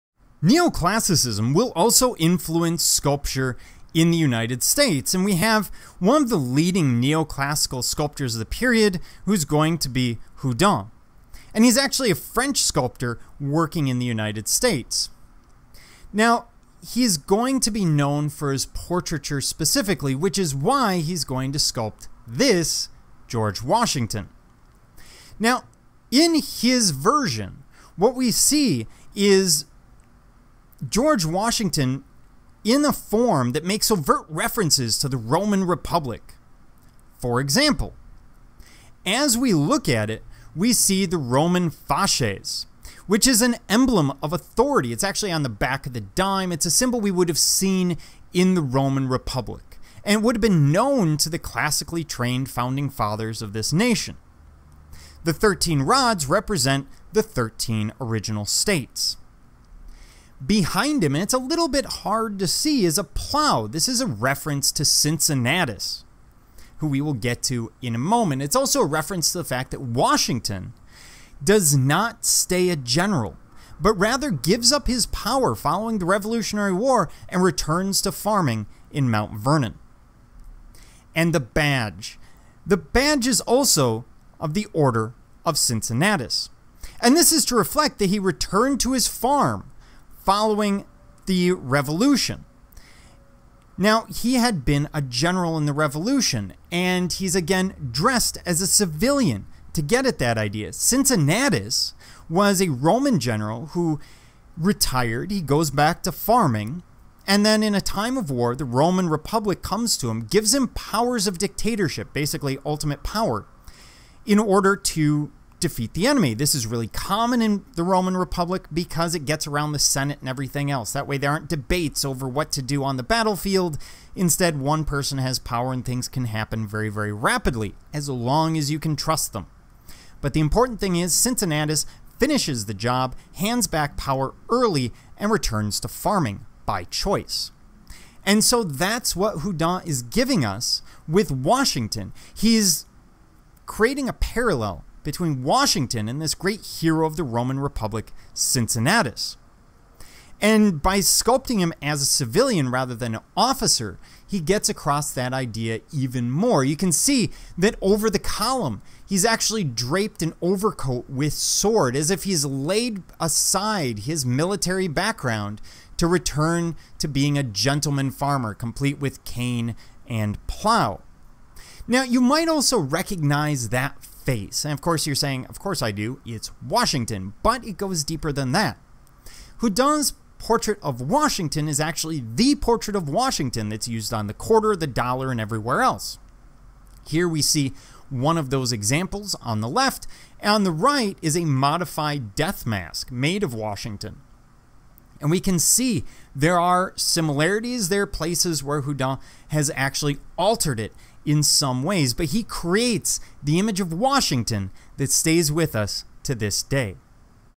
Houdon George Washington
A brief look at Houdon's sculpture of George Washington